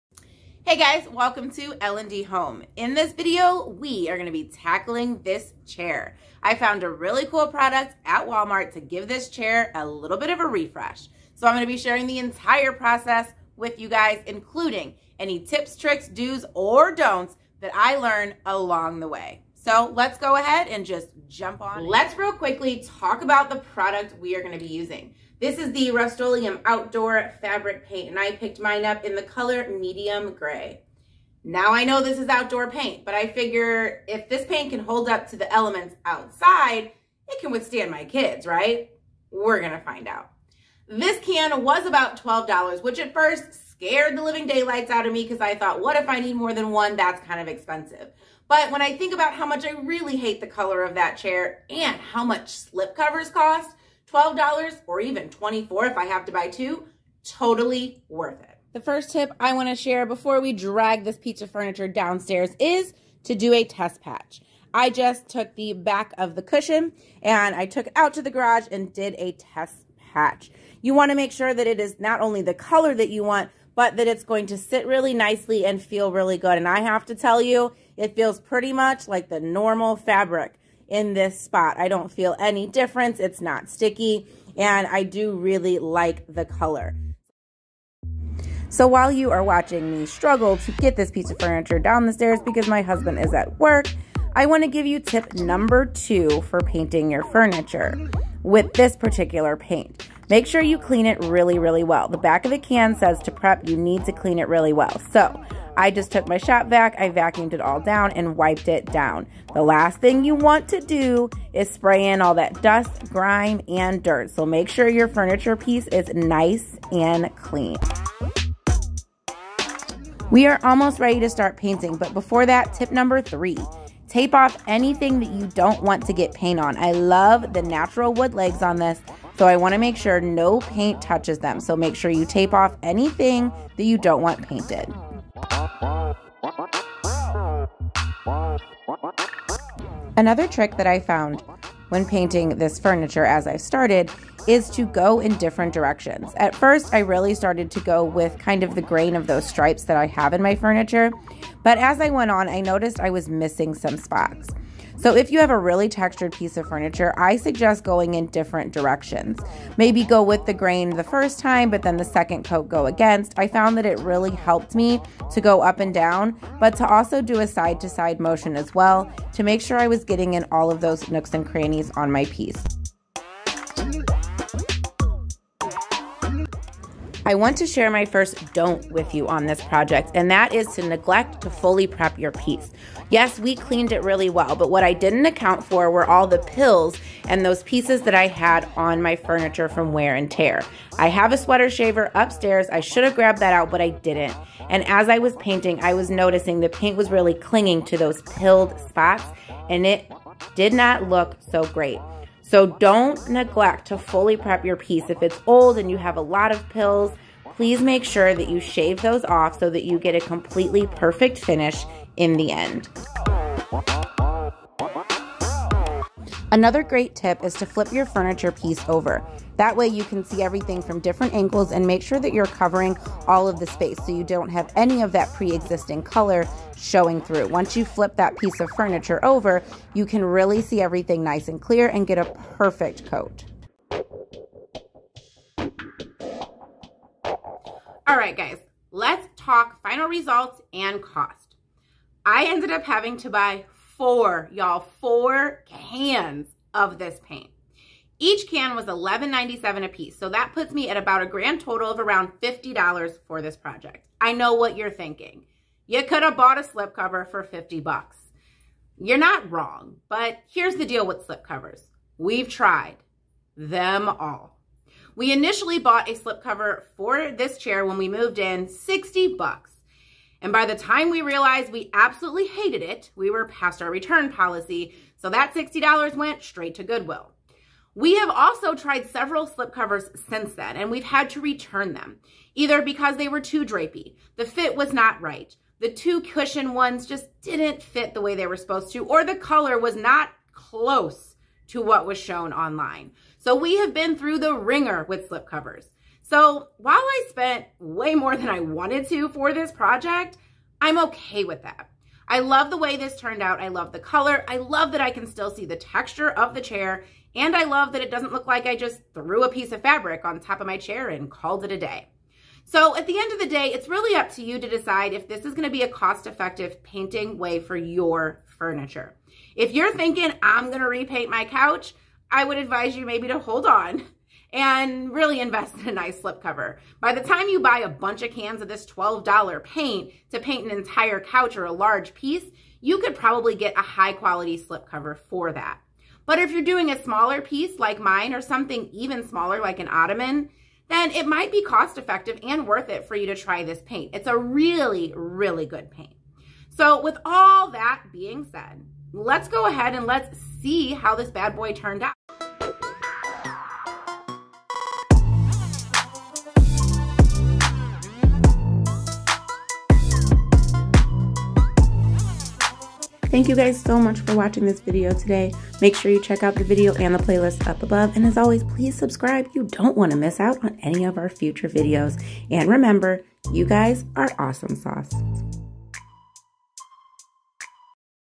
PAINTING FABRIC FURNITURE | How to PAINT FABRIC
Painting your fabric furniture can be scary! In today's video, I share do's and dont's I learned from my first time painting a fabric chair. I also share my thoughts on the end an result and if I think it's worth the cost and effort. Enjoy this furniture flip!

      landdhome
* Send us Mail
       7055 Mexico Rd. 457​​